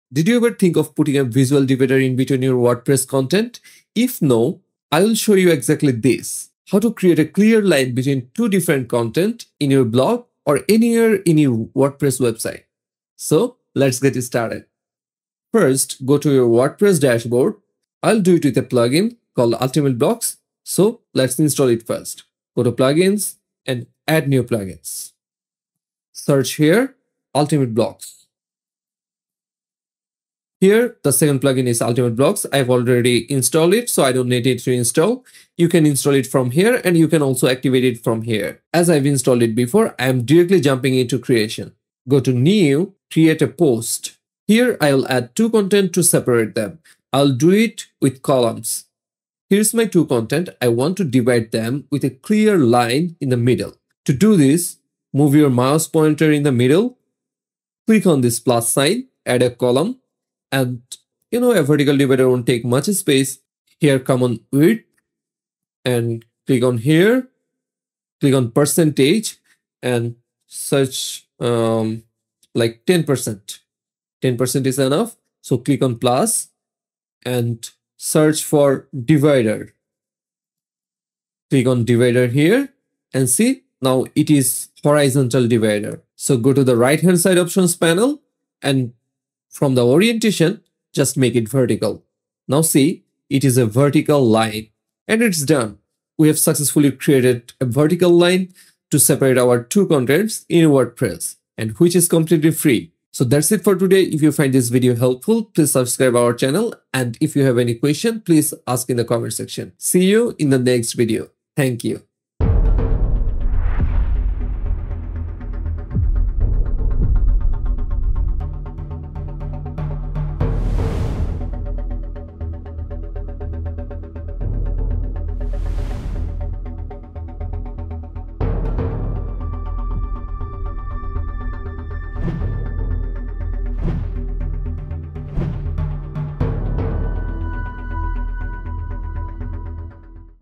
How to Add a Vertical Line Separator in WordPress
Learn how to add a vertical line separator in WordPress using the Ultimate Blocks plugin.